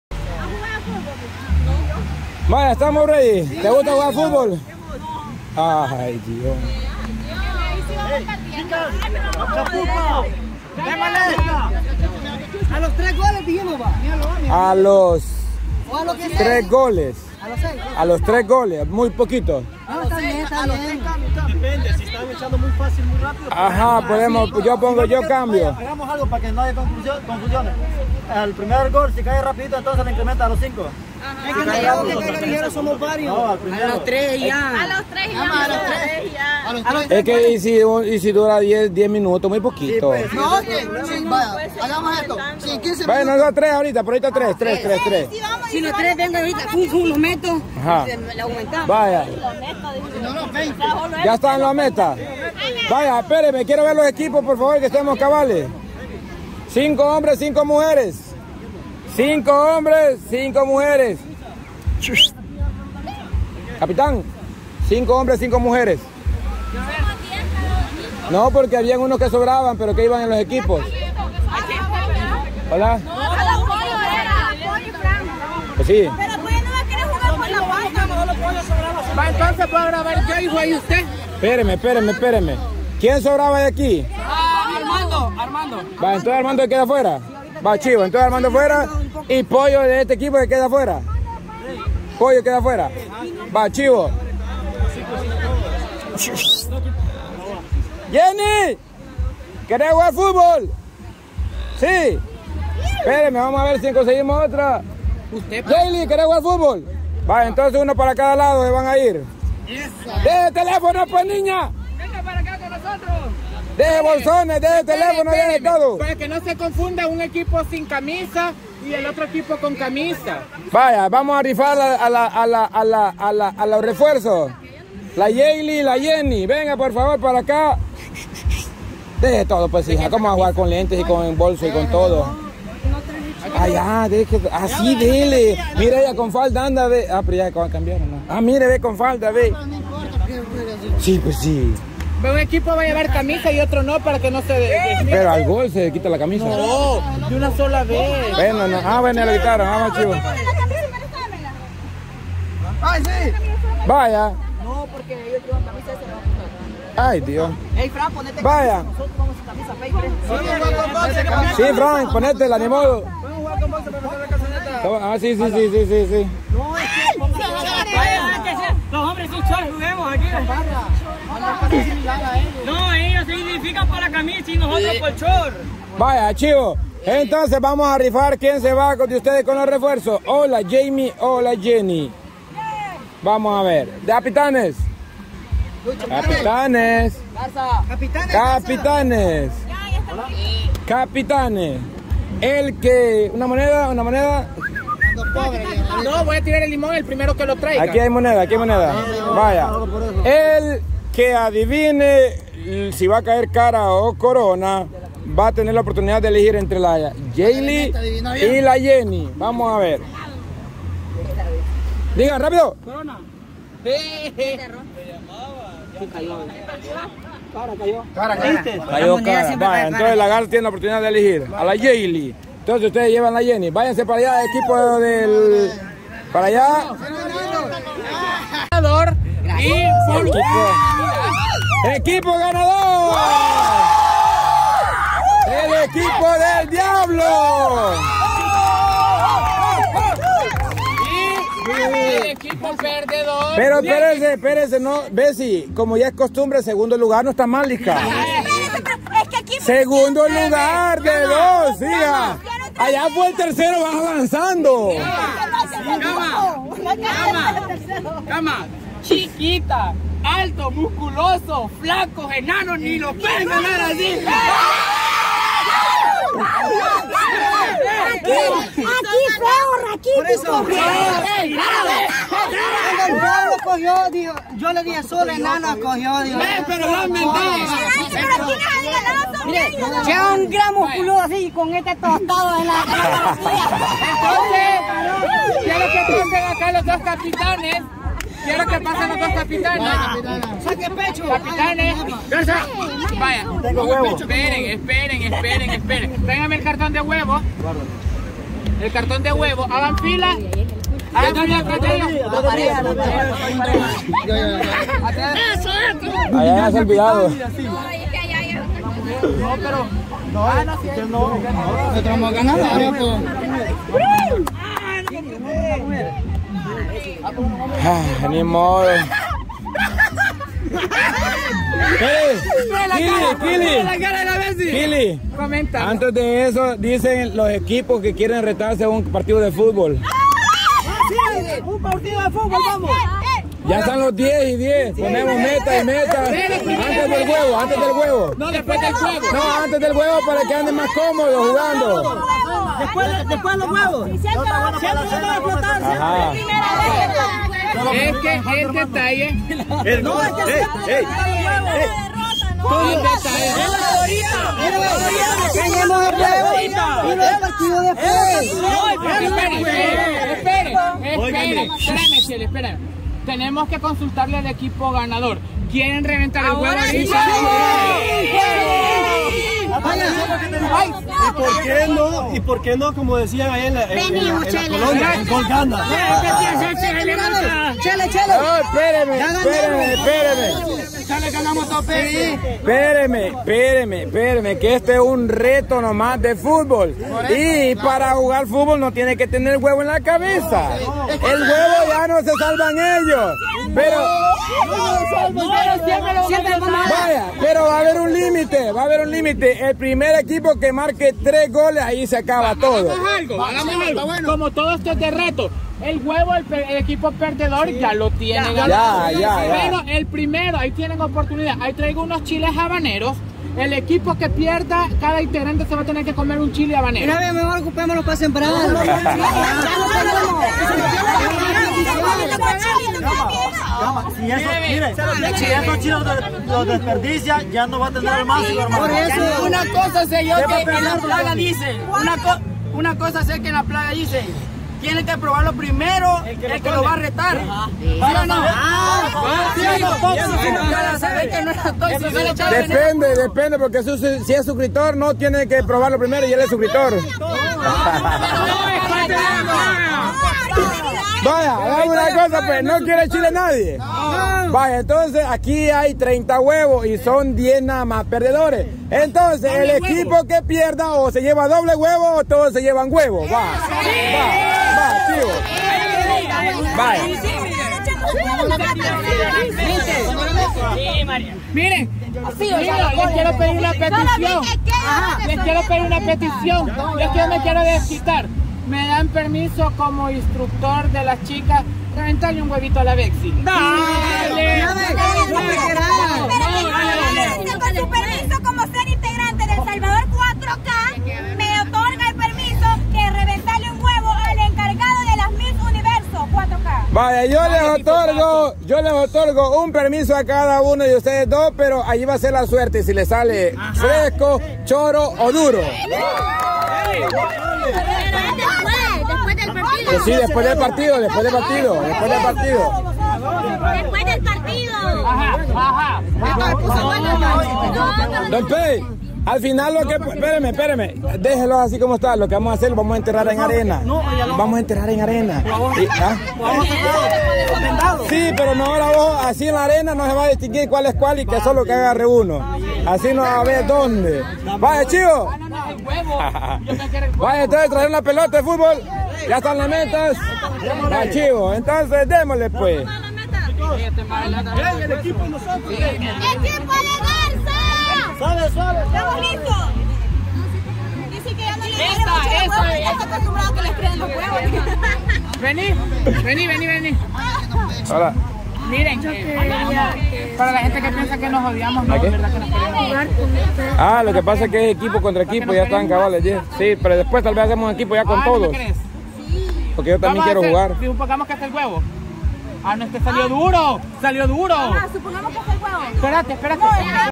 Bessy sigue enojada con todos por haber perdido y se agarró con Kily. Listos para el reto de futbol.
Visita nuestra tienda Oficial y ordena la mercaderia que te guste